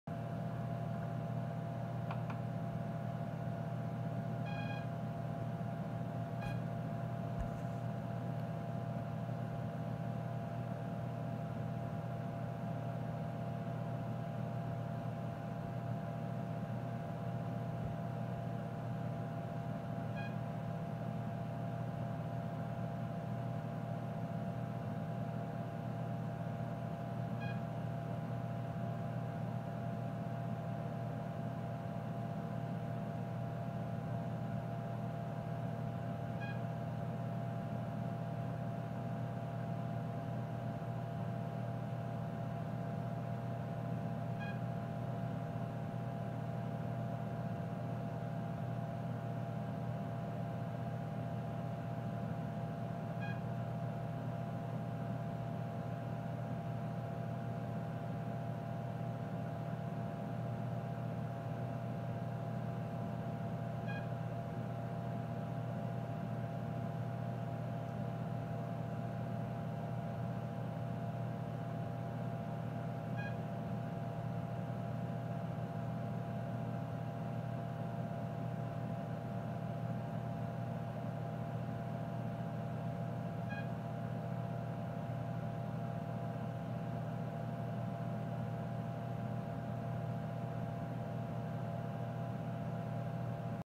Tektronix 4051/4052/4054 Factory Test
Display test used in the 1980’s by Tektronix on their 4050 series computers.  This is a video of my 4054A computer running this test program.